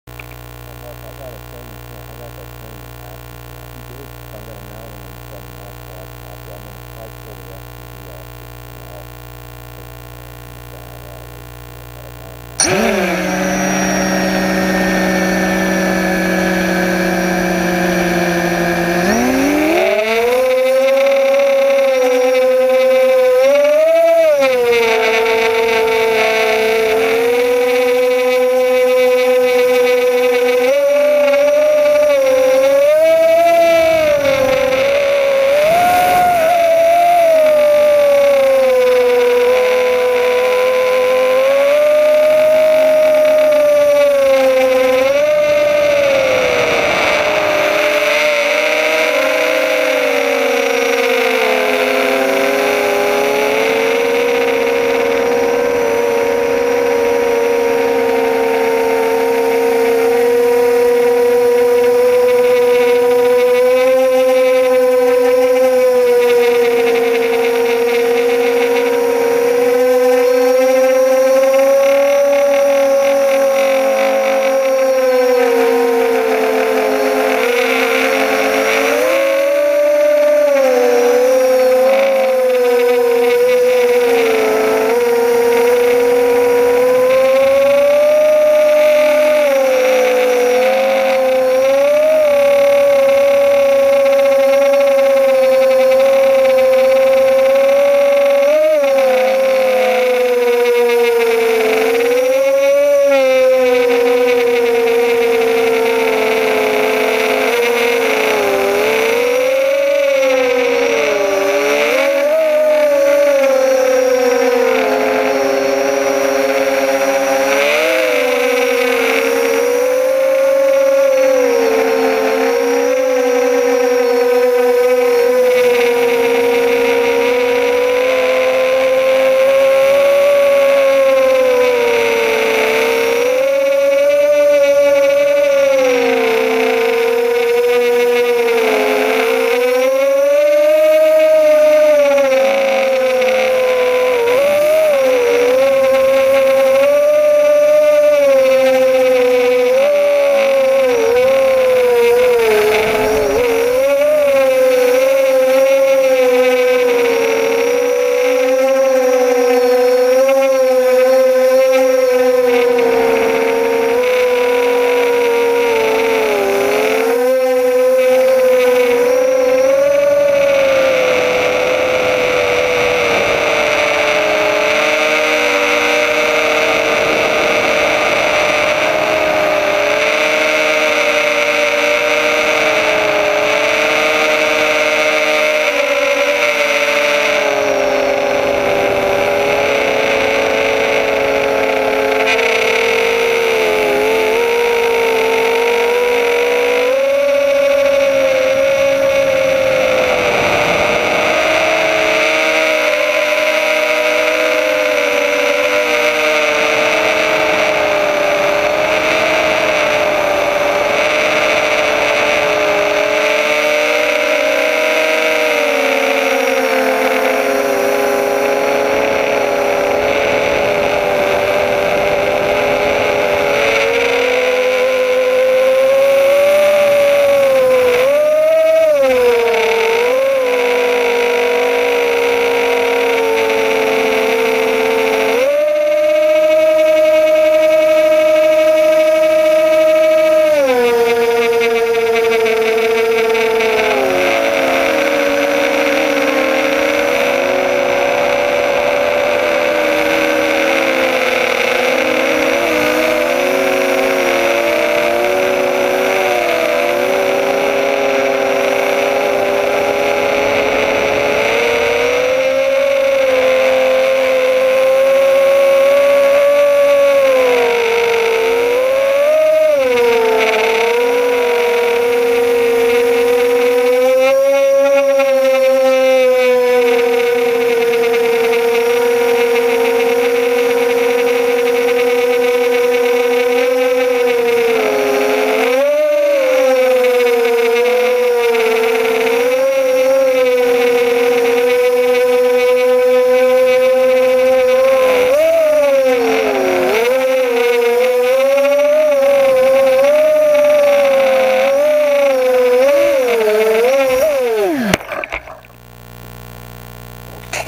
Sept 21, 2014 - flight one Blackout FPV
First flight of the day. Foggy morning with a little rain mixed in. Blackout mini h quad videoing Chris's 3D biplane.

Uncut video